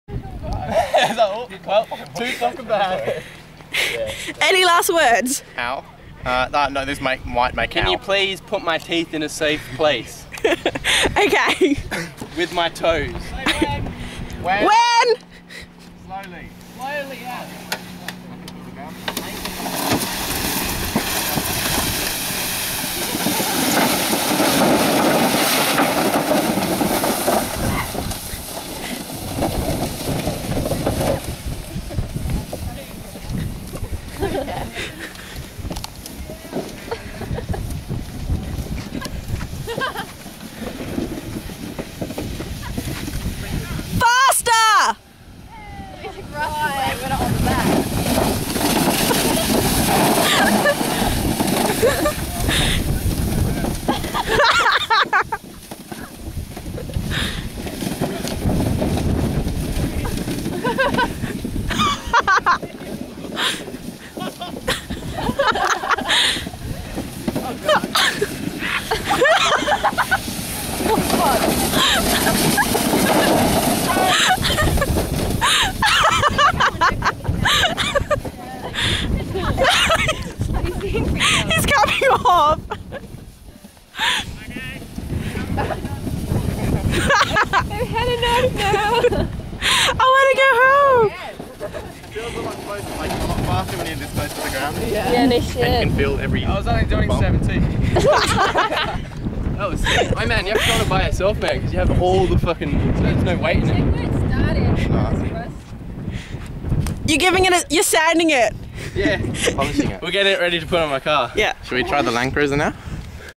Bonnet Riding
Cameron and Joel go bonnet riding.